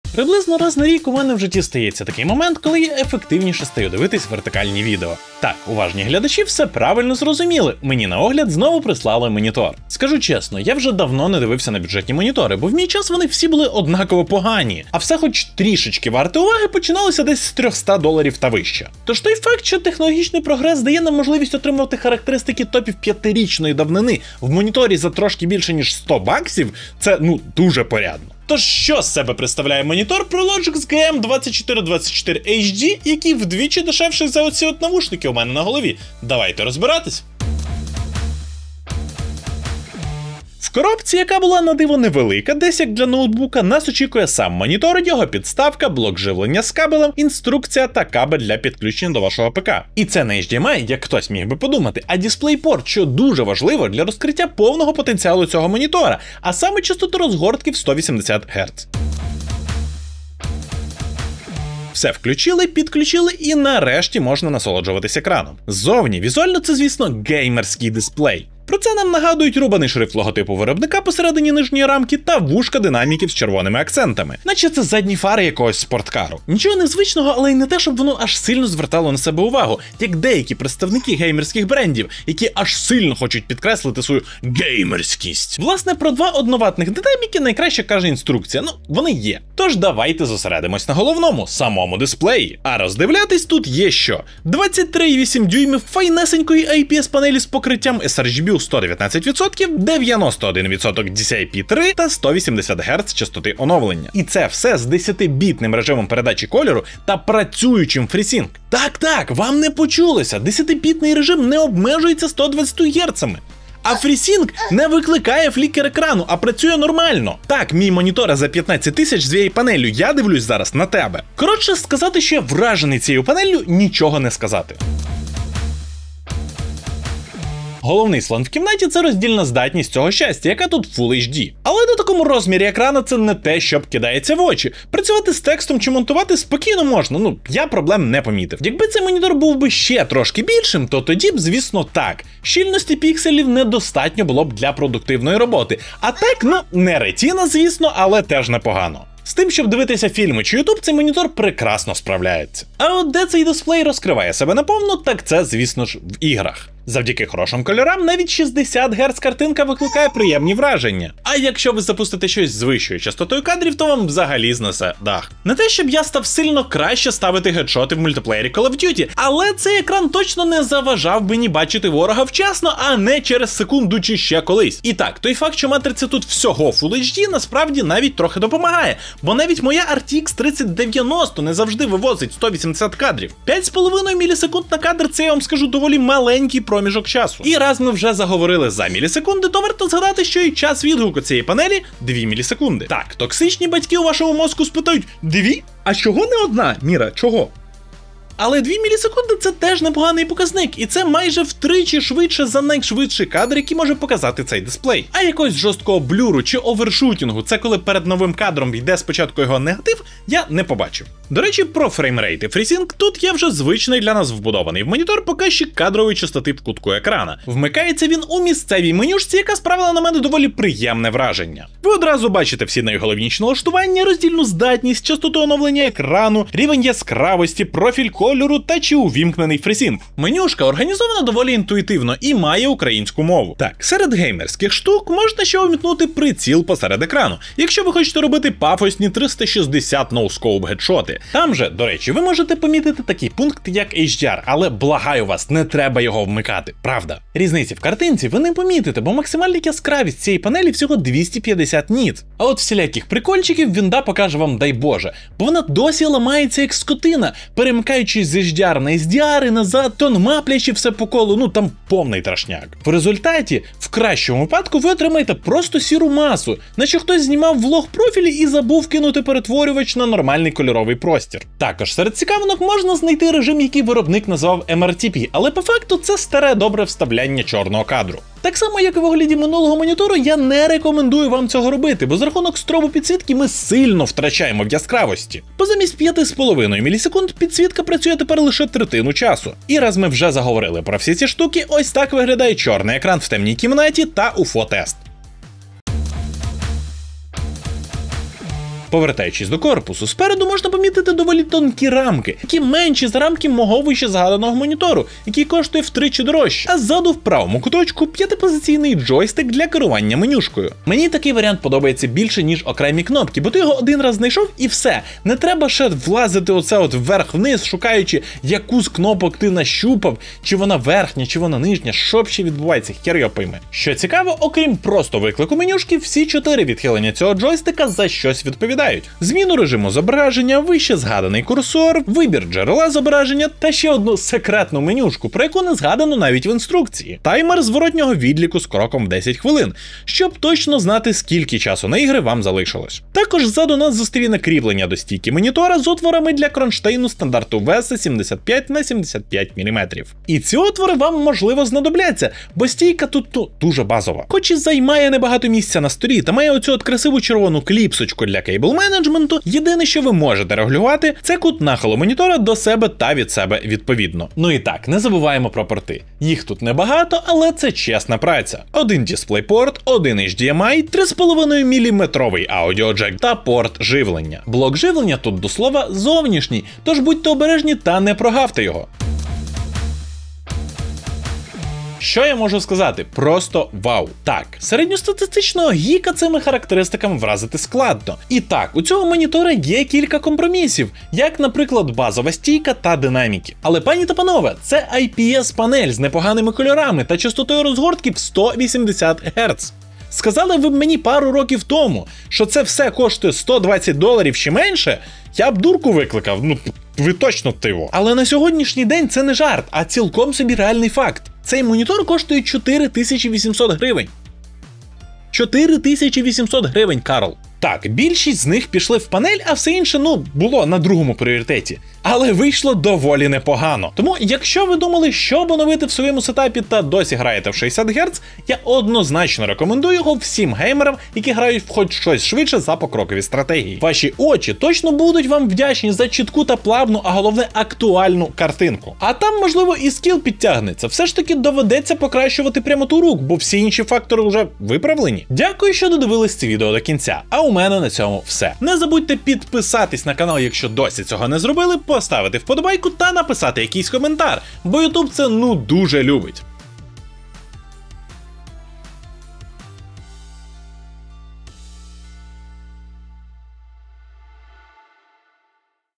І ЦЕ тепер бюджетний монітор? — Огляд монітора Prologix GM2424HD
Той факт що технологічний прогрес дає нам можливість отримувати характеристики топів 5 річної давнини в моніторі за трошки більше ніж 100 баксів — це порядно. Що з себе представляє монітор Prologix GM2424HD вдвічі дешевший за навушники що зараз у мене на голові? Давайте розбиратись.

Над відео працював: Володимир Бортюк

Дякуємо за підтримку! 🙂💪